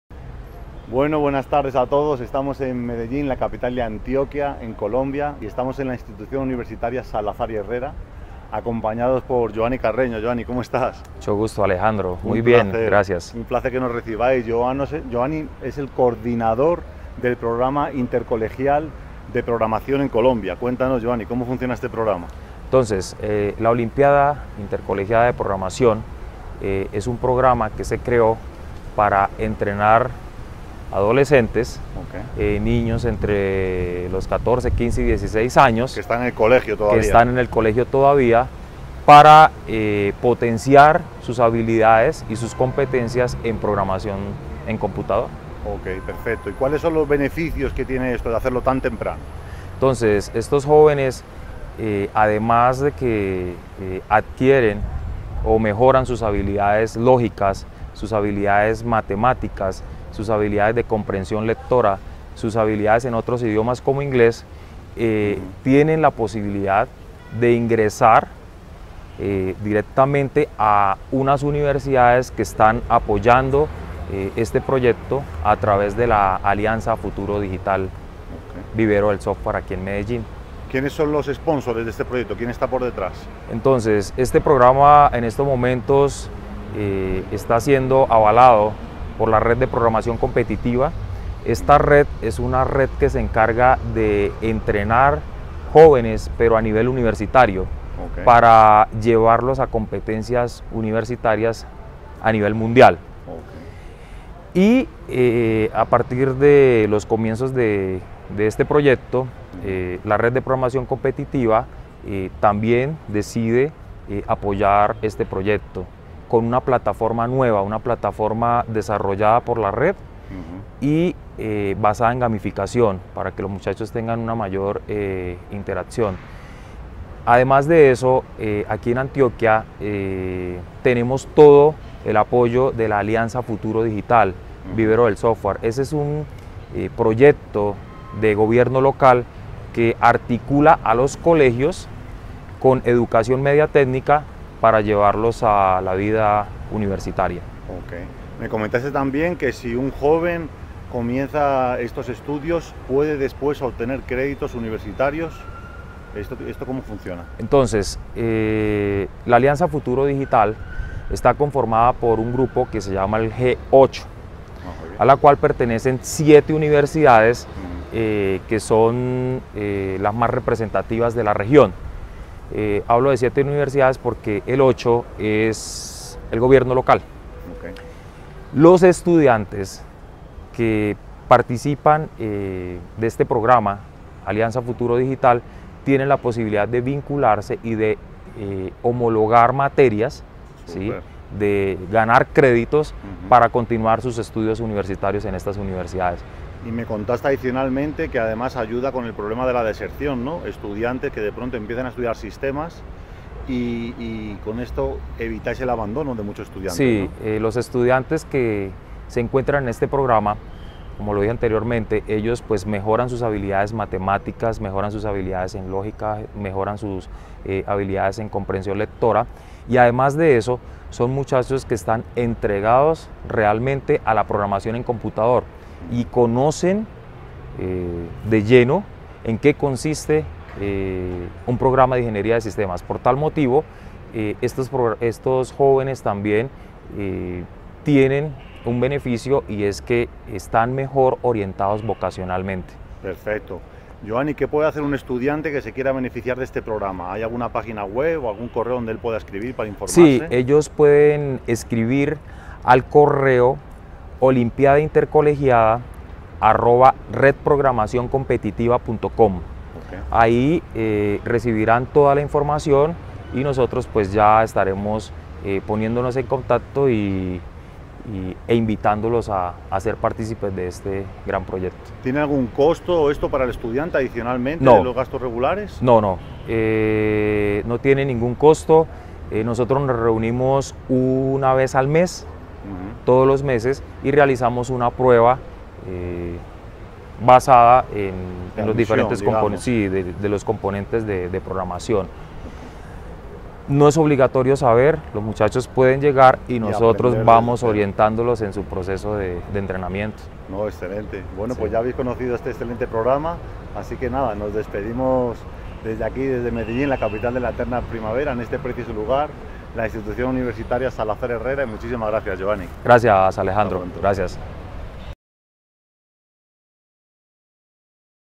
Olimpiada Intercolegiada de Programación
Entrevistamos a Johany Carreño (@EstrategaEduTIC), uno de los organizadores de la Olimpiada Intercolegiada de Programación.

Se trata de una competencia algorítmica y de programación dirigida a estudiantes de educación media que persigue principalmente estimular el estudio de las Ciencias de la Computación y la Informática.

Este evento se realiza vía Red de Programación Competitiva – RPC y cuenta con el respaldo de la Alianza Futuro Digital de Medellín con el fin de fortalecer el talento TI en Colombia.